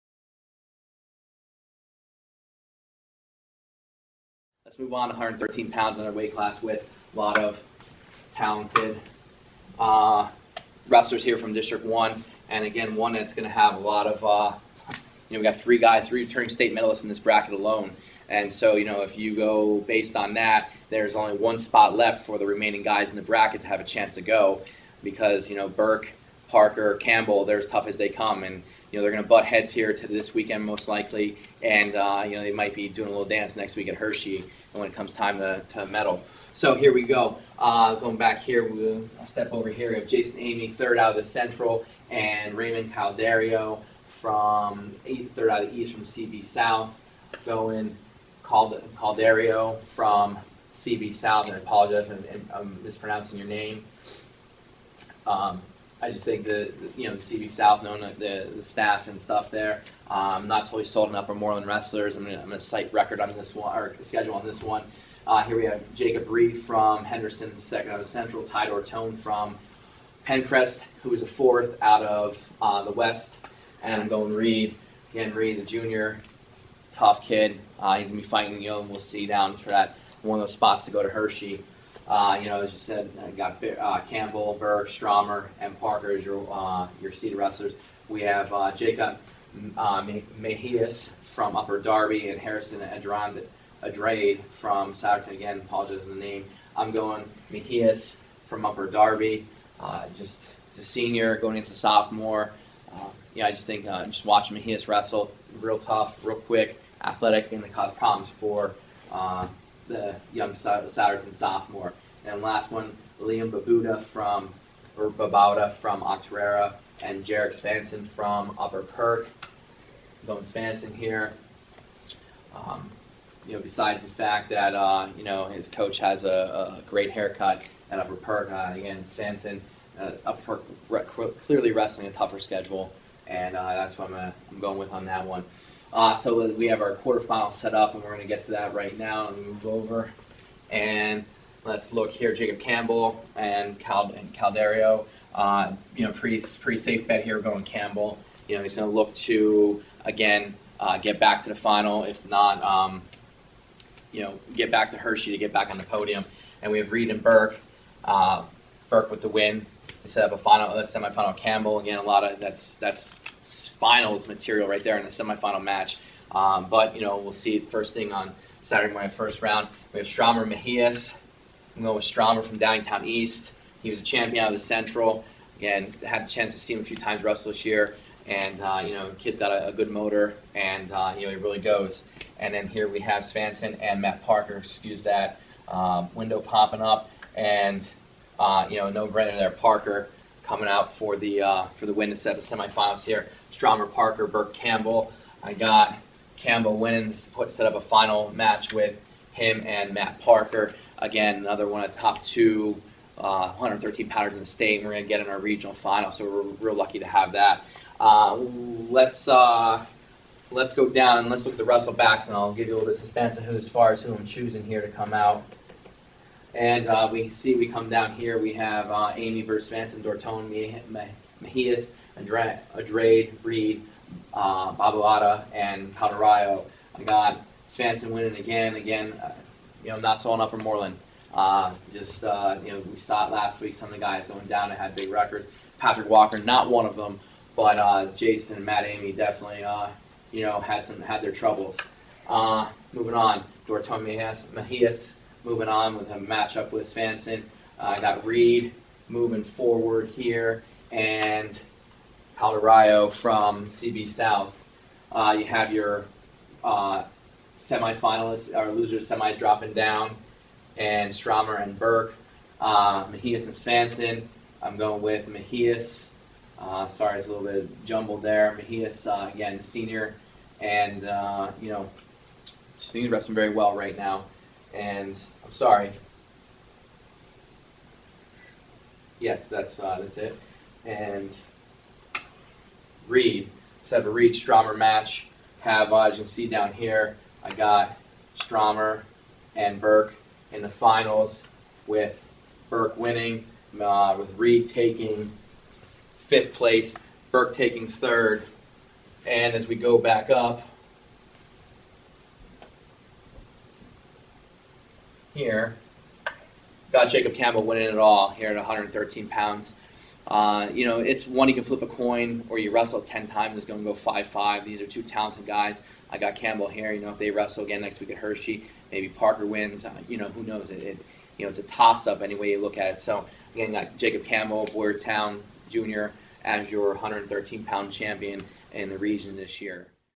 2016-03-02 SE AAA Region 113 pound preview and predictions
PA-Wrestling.com's Joe Youngblood previews the 2016 PIAA South East AAA Regional wrestling tournament and gives his predictions for how the brackets will play out this Friday and Saturday at Souderton HS.